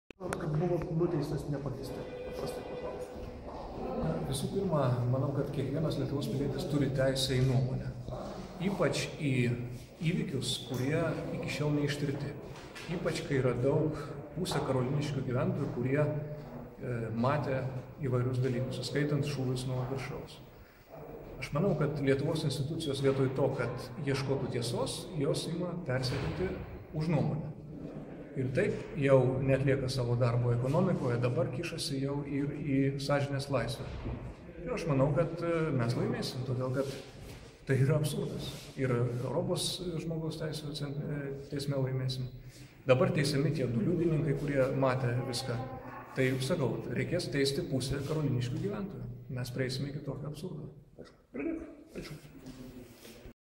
15min.lt - A.Paleckio komentaras dėl Sausio 13-osios bylos Aukščiausiajame Teisme
Išplėstinė (septynių teisėjų) Lietuvos Aukščiausiojo Teismo (LAT) kasacine tvarka nagrinėja Algirdo Paleckio baudžiamąją bylą dėl Sausio 13-osios nusikaltimų neigimo.
A.Paleckis reikalauja panaikinti ankstesnį jau įsiteisėjusį Vilniaus apygardos teismo nuosprendį, kuriuo jis pripažintas kaltu dėl viešai pasakytų žodžių apie 1991 m. sausio 13 d. įvykius prie televizijos bokšto, esą „saviškiai šaudė į savus." Anot A.Paleckio, Apygardos teismas neatsižvelgė į jo ir advokato argumentus, neįvertino nuteistajam palankių įrodymų.
LAT skelbs verdiktą 2013 m. sausio 22 d.
2012 m. gruodžio 11 d. po posėdžio A.Paleckis pateikė komentarą žiniasklaidai.